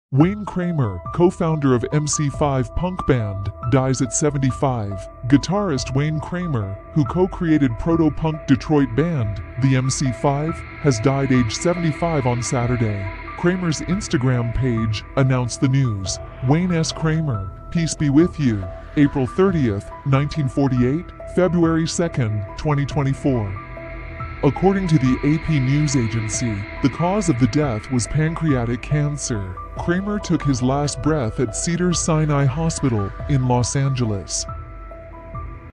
Wayne Kramer, co-founder of MC5punk band, dies at 75😱😱todaynews
Full News 🗞️🗞️🗞️🗞️
Wayne Kramer, co-founder of MC5 punk band, dies at 75.

Guitarist Wayne Kramer, who co-created protopunk Detroit band the MC5, has died aged 75 on Saturday. Kramer's Instagram page announced the news: “Wayne S Kramer. Peace be with you. April 30, 1948 – February 2 2024".

According to the AP news agency, the cause of the death was pancreatic cancer. Kramer took his last breath at Cedars-Sinai Hospital in Los Angeles.